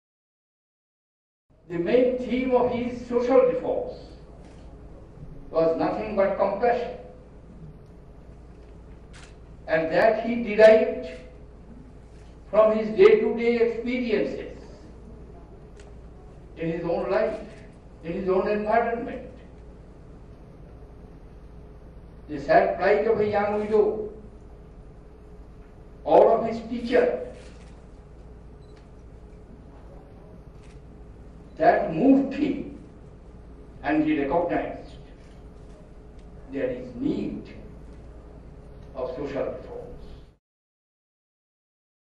President Pranab Mukherjee at 7th Vidyasagar Memorial Lecture
Delivering the 7th Vidyasagar Memorial Lecture at the Vidyasagar University President Pranab Mukherjee said "reforms in police and judiciary need to be taken up on a continuing basis and in all earnest so that women get fair and prompt justice. These measures should be further backed by efforts aimed at empowerment of women and improvement of their health, education and employment opportunities."
"At a time when our country is witnessing the sad spectacle of people including women and children being killed in communal clashes, the life of Pandit Vidyasagar teaches us that religion should never become an instrument for propagation of hatred and violence against believers of different faiths," the President said.